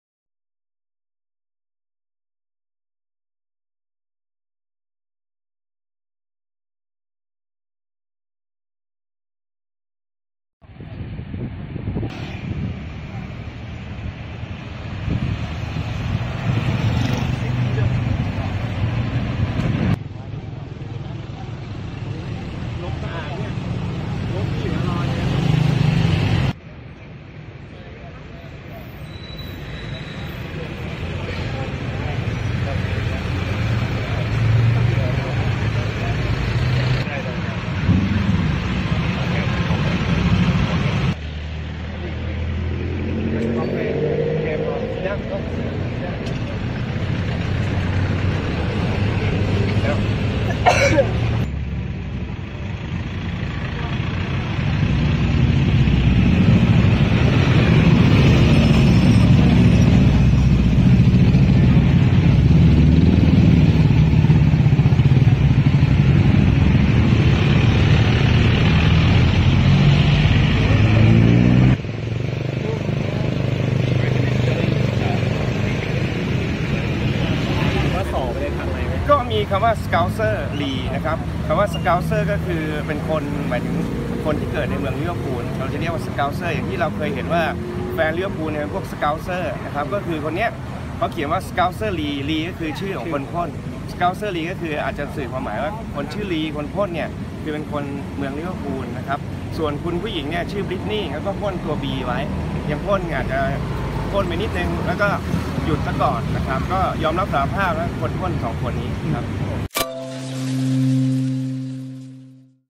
Briton and Canadian face prison after spraying 'Scousse Lee' on 800-year-old Thai fortress
A backpacker from Liverpool was facing jail today (October 19) after spraying ''Scousse Lee'' on an 800-year-old Thai fortress.

Lee Furlong, 23, was caught on CCTV using a can of black spray paint to tag the historic Tha Phae gate in Chiang Mai, northern Thailand, on October 18 at 4am.

The graffiti – which read ''Scousse Lee'' and ''B'' on the line below – sparked outrage from locals who accused the traveller of disrespecting the ancient 13th-century monument and their country.

Police launched a manhunt and Furlong, as well as Canadian woman Brittney Schneider, 23, who was with him at the time and sprayed ''B'', were arrested yesterday afternoon.

Officers frogmarched them to the site – which had already been cleaned – where they confessed to the crime. The pair now faces up to 10 years in prison and/or a one million baht (25,000gbp) fine for desecrating a historical site.

Lieutenant Colonel Teerasak Sriprasert from Chiang Mai police said: ''The graffiti says ''Scousse Lee''. This means ''Scouser Lee from Liverpool''. The girl is called Brittney and she wrote a letter 'B' on the wall.

''Officers investigated the vandalism after it was seen on CCTV cameras. The offenders were tracked to a guest house near the same road as the wall.

''The accused will be investigated and prosecuted according to the law.''

Lieutenant Colonel Teerasak took the pair to the wall yesterday where they were seen standing in front of the crime scene and pointing to where the graffiti was.

He added: ''The accused were questioned and confessed that the incident happened earlier in the morning at 4am.''

Cops said that Furlong and Schneider told them they had been drinking with friends at a nearby restaurant until they were drunk.

They were walking back to the Mad Monkey Hostel when they found the spray paint lying on the ground and decided to graffiti the wall as a ''prank''.

Furlong is seen spraying ''Scousse'' before the Canadian girl puts the first initial of her name below. The Liverpudlian then finished the tag off with his name ''Lee''.

The pair only stopped when a passing tuk-tuk driver intervened and told them off, at which point they left the spray can and walked back to the hostel.

Residents reported the crime at first light and it took police just a couple of hours to track down the pair.

They now face prosecution under Thailand's Antiques, Objects of Art and National Museums act 1961, section 32, which carries a maximum punishment of ten years in prison and/or a one million baht fine for ''anyone who invades an ancient site, or damages, destroys, degrades or renders it useless''.

Tha Phae gate is part of a crumbling wall was built in the 13th century as a fortress to protect the city of Chiang Mai. It is the centre of tourism in the bustling northern city popular with backpackers and is surrounded by bars, restaurants, hotels, markets, massage parlours and shops.
Play Video
Play
Current Time
0:00
Loaded: 0%
Progress: 0%
Duration Time
1:52
Mute
Quality
720p
HD
Fullscreen